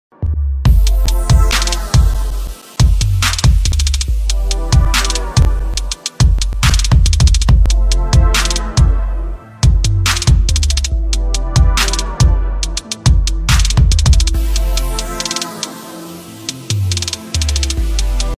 Anti Gravity Remote Control Car
🎮🏎️The Remote Control Car That Defies Gravity!
Drives up any smooth wall☑️ Drives on the ceiling☑️